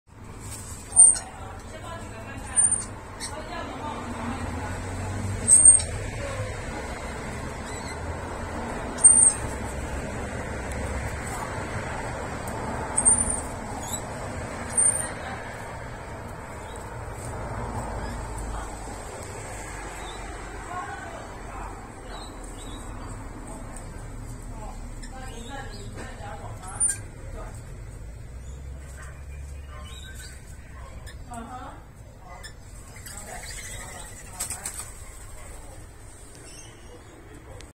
虹彩吸蜜鸚鵡與茶花20 (Trichoglossus haematodus)
本來以為只有兩隻彩虹吸蜜鸚鵡，等到吃飽飛走才發現樹裡面還藏著第三隻。